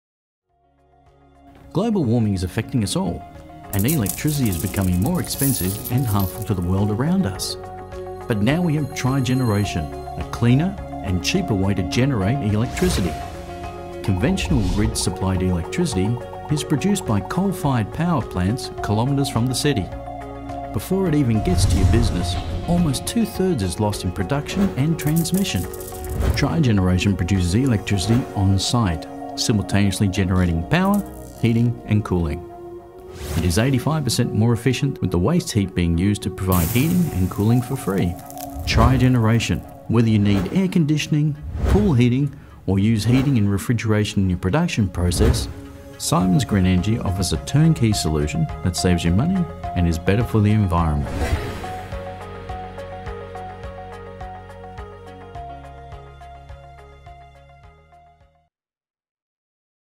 What is Trigeneration and how does it work?
Global warming is affecting us all, and electricity is becoming more expensive and harmful
to the world around us.

But now we have Trigeneration! A cleaner and cheaper way to generate electricity.

Conventional grid supplied electricity is produced by coal fired power plants kilometers from the city.

Before it even gets to your business almost  2/3rds) is lost in production and transmission.

Trigeneration produces electricity onsite, simultaneously generating POWER, HEATING
AND COOLING.

It is 85% more efficient, with the waste heat being used to provide heating and cooling for Free

Whether you need air conditioning, pool heating or use heating and refrigeration in your production process, Trigeneration will help your business save money and reduce its carbon footprint.

Trigeneration! It's sustainable, saves you money and is better for the environment.